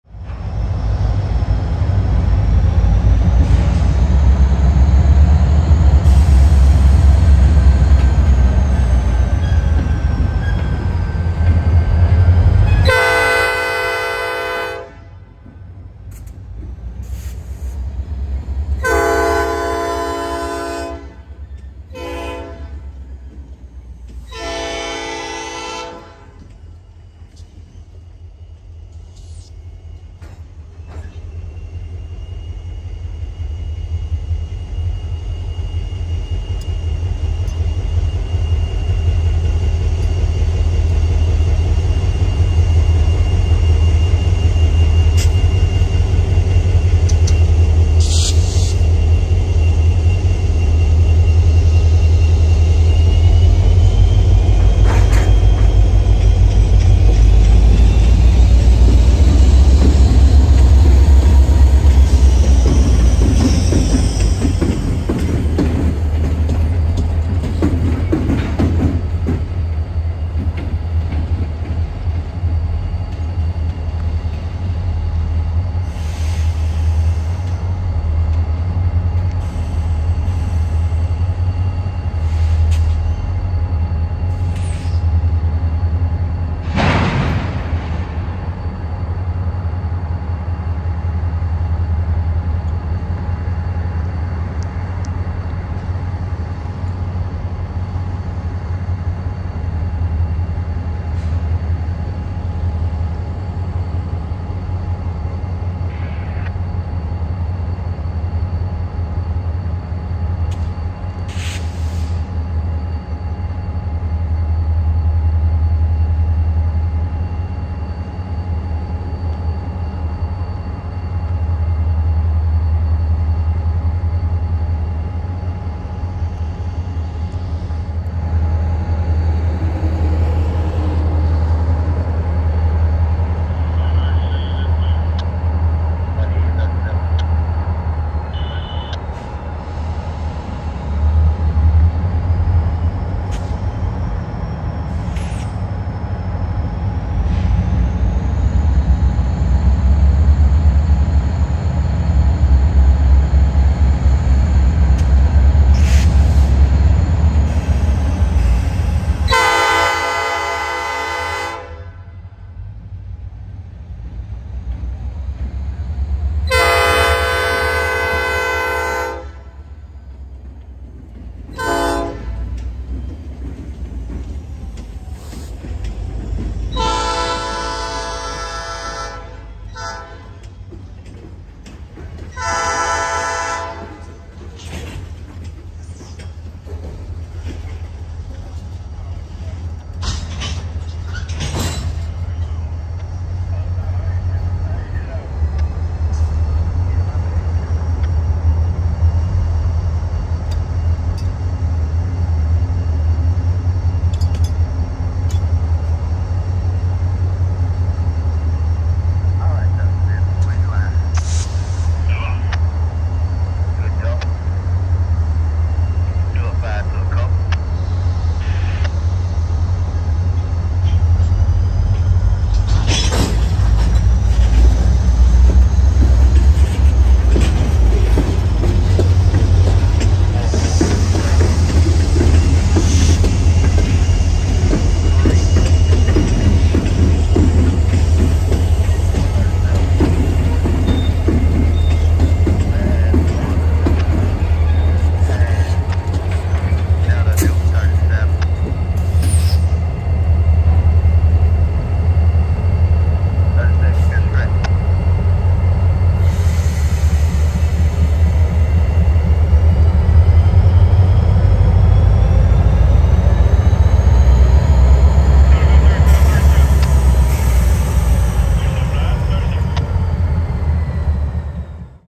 NS SD40-2 3218 Does Some Switching on Local A37-11 on 1/11/24
Train: A37-11 (Tuscaloosa - Dudley, AL turn)
Location: 12th Avenue, Tuscaloosa, AL
District: AGS South

NS 3218- SD40-2 (NC P5)